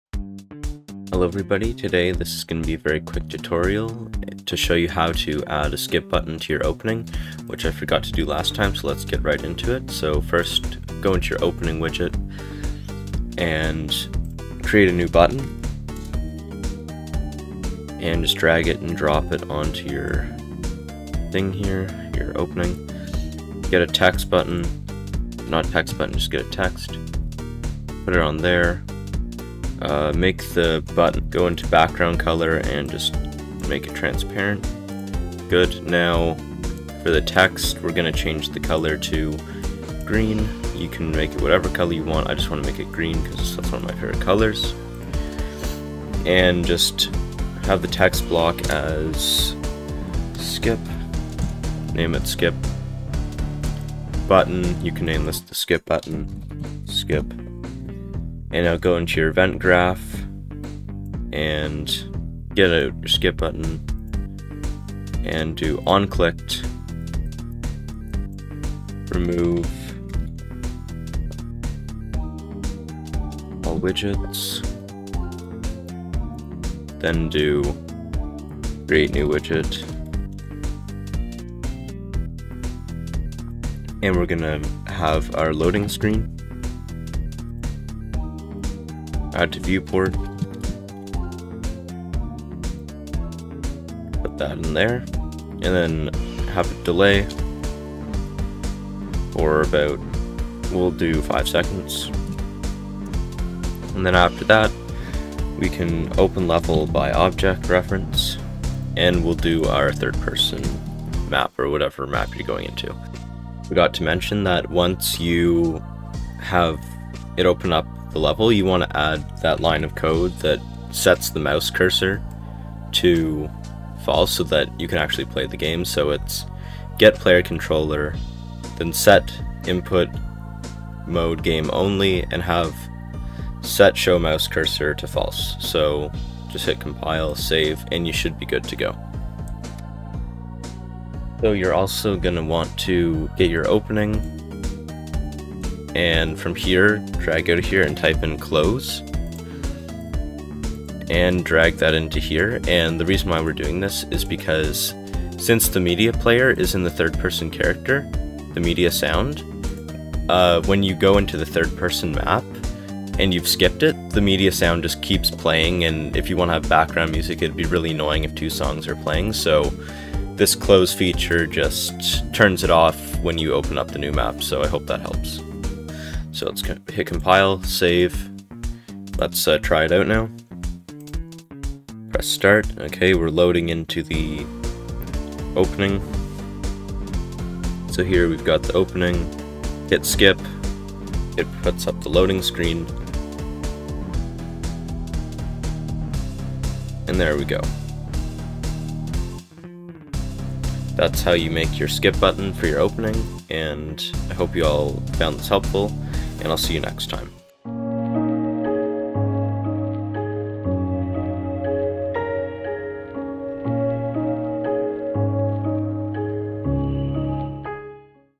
Adding a Skip Button to Your Opening Video (Unreal Engine 5.1)
In this fast tutorial I will show you how to add a skip button to your opening in unreal engine 5.1